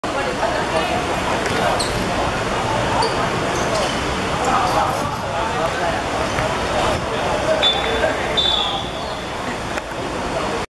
太陽國際vs明興(2015.8.12.九龍青少年盃U16足球賽)之入球1:0
烏龍波